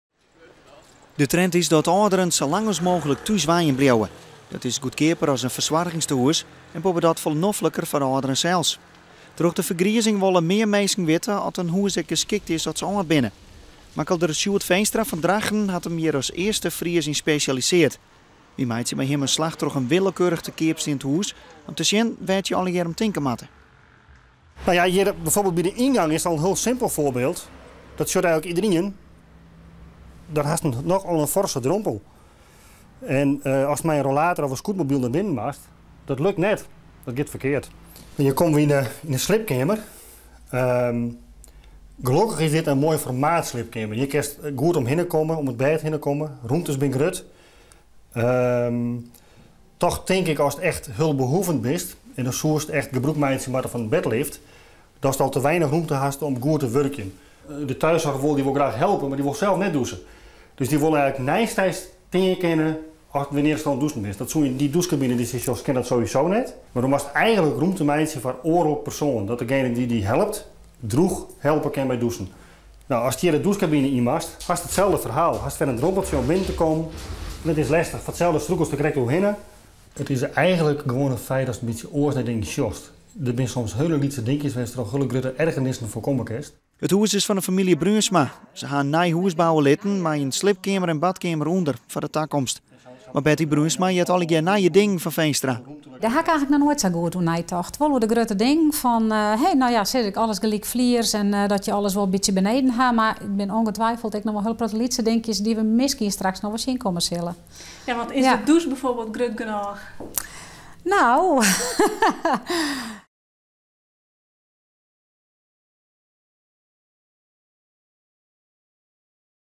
Earste senioaremakelder los
Sjoerd Veenstra út Drachten is de earste Fryske makelder mei it NVM-karmerk senioaremakelder. Dizze makelders binne saakkundich om de âlderein te helpen by it sykjen nei in geskikte wente of ien dy't geskikt makke wurde kin. Dat is nedich as de minsken problemen mei har sûnens krije.

Tefolle senioaren keapje in hûs dat net geskikt is, omdat der net goed neitocht wurdt oer wat der yn de takomst barre kin. Sels guon senioarewenten foldogge net oan de easken en dat komt omdat sels arsjitekten net altyd op de hichte binne fan wat nedich is.